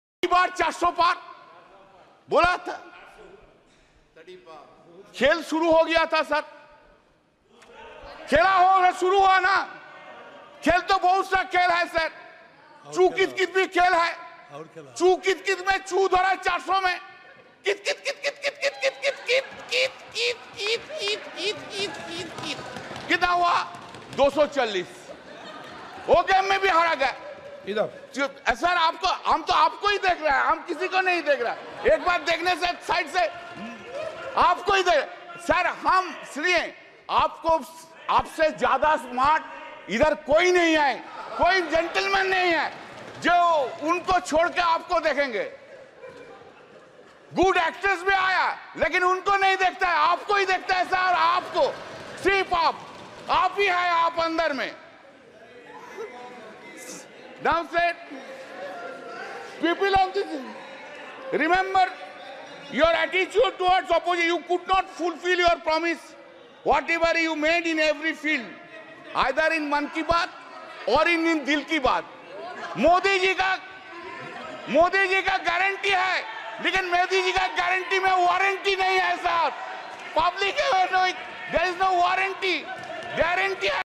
TMC MP Kalyan Banerjee Speech Lok Sabha: लोकसभा में कल्याण बनर्जी ने की ऐसी एक्टिंग, जमकर लगे ठहाके
लोकसभा में आज TMC सांसद कल्याण बनर्जी ने BJP और मोदी सरकार पर जोरदार तंज कसा। इसके लिए उन्होंने कित, कित, कित, कित नाम के एक खेल और अपनी एक्टिंग का सहारा लिया। लोकसभा में राष्ट्रपति के अभिभाषण पर धन्यवाद प्रस्ताव पर चर्चा में शामिल होते हुए कल्याण बनर्जी ने कित-कित खेल का जिक्र किया। इस दौरान उनकी एक्टिंग देख कर पूरा सदन ठहाकों से गूंज उठा। कल्याण बनर्जी की एक्टिंग देख तो उनकी ही पार्टी की सभी महिला सांसद हंस पड़ीं।

In the Lok Sabha today, TMC MP Kalyan Banerjee took a strong dig at BJP and Modi government. For this he took the help of a game named Kit, Kit, Kit, Kit and his acting. While participating in the discussion on the motion of thanks on the President's address in the Lok Sabha, Kalyan Banerjee mentioned several sports. During this, the whole house erupted in laughter after seeing his acting. Seeing Kalyan Banerjee's acting, all the women MPs of his own party laughed.